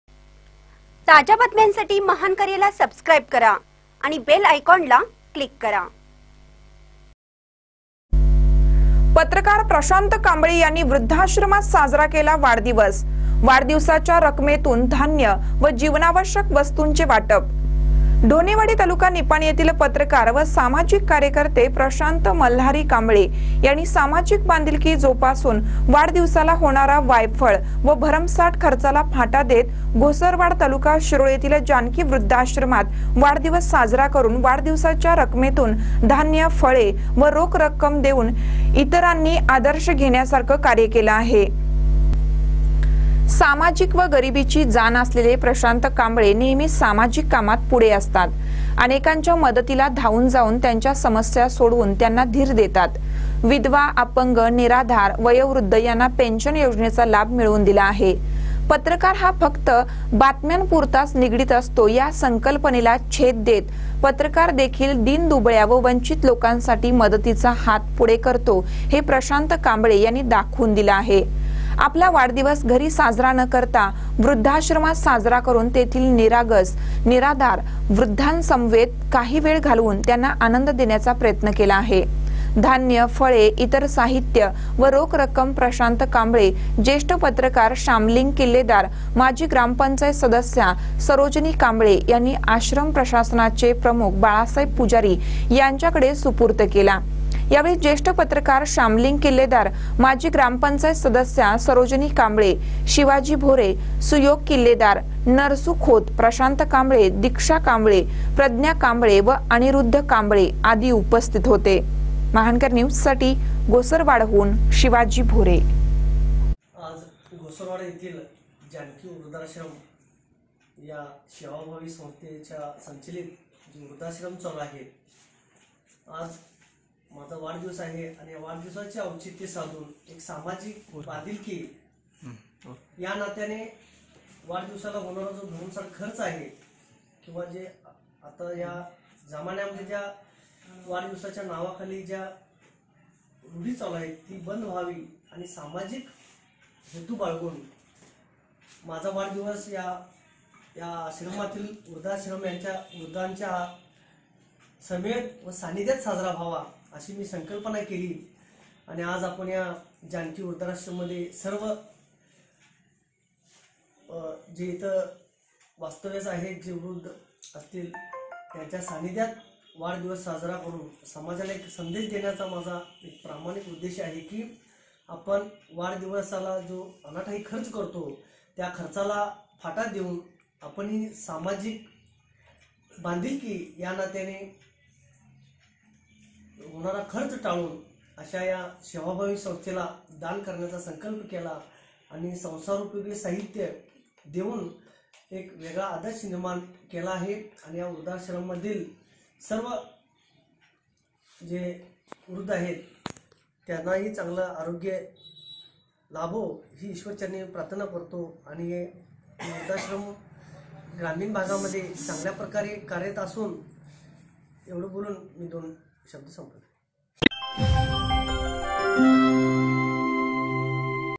पत्रकार प्रशांत कांबळे यांनी वृद्धाश्रमात साजरा केला वाढदिवस
पत्रकार प्रशांत कांबळे यांनी वृद्धाश्रमात साजरा केला वाढदिवस, वाढदिवसाच्या रक्कमेतून धान्य व जीवनावश्यक वस्तूंचे वाटप